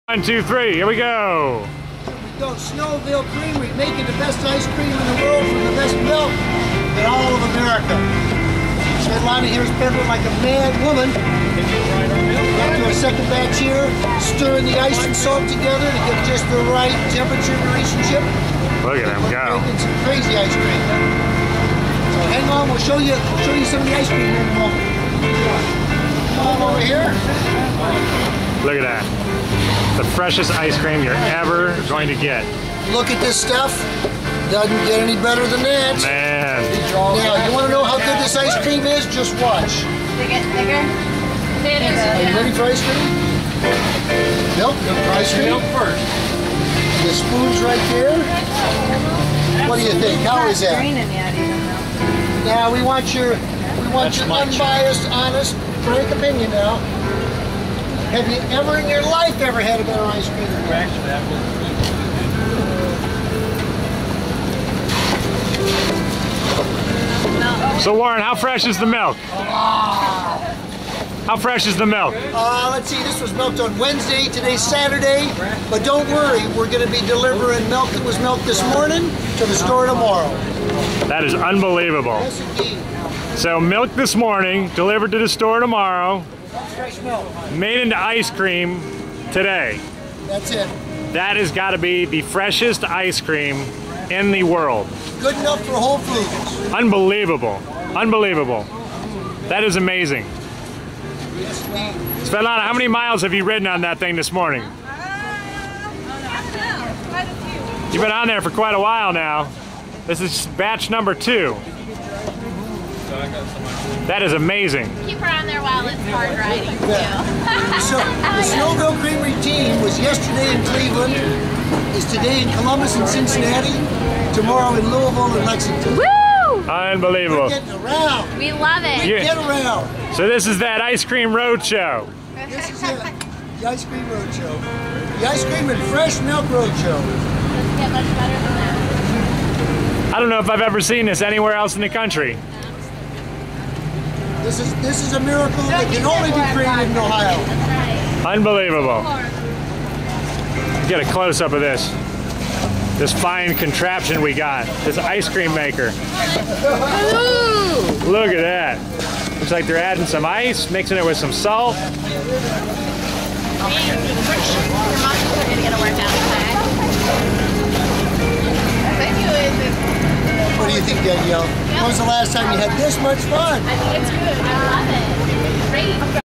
Cincinnati WFM and Snowville Creamery- fresh made ice cream
Svetlana making ice cream and earning Earth Trek points